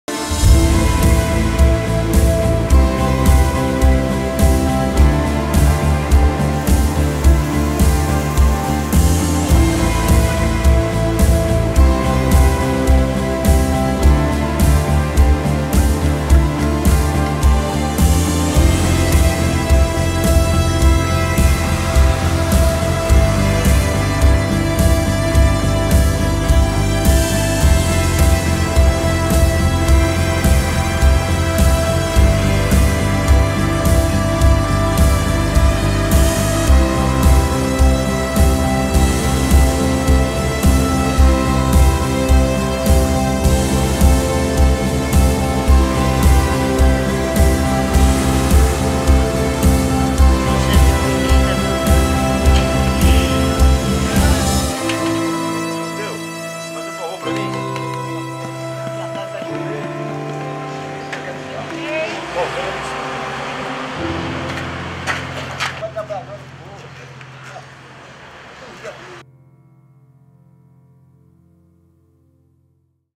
'Cuidando de Santos': veja os serviços realizados hoje (2) e a agenda de quinta (3)
Mutirão de zeladoria tem percorrido a Cidade para desobstruir galerias de água e retirar o mato das vias públicas.